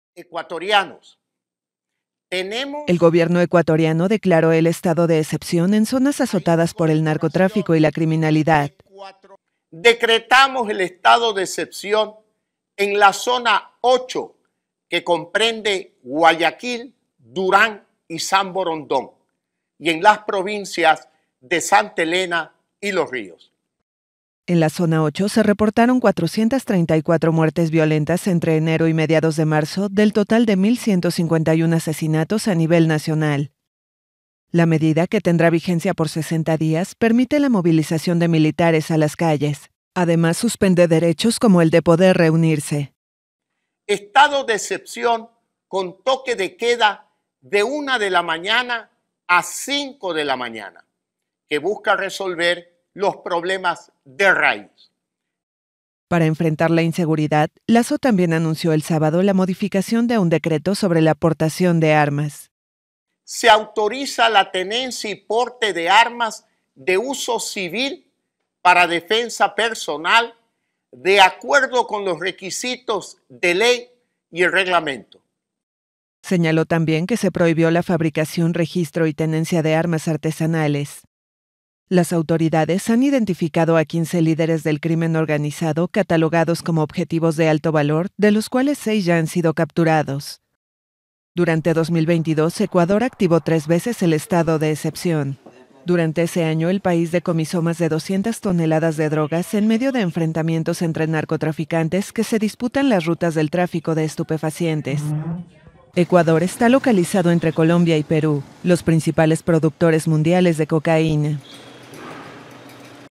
Ecuador declara estado de excepción en zonas golpeadas por el narco
Ecuador declaró el sábado el estado de excepción en varias zonas azotadas por el narcotráfico y la criminalidad como el puerto de Guayaquil (suroeste), donde esa medida que permite movilizar militares a las calles ha sido dictada en tres ocasiones durante 2022.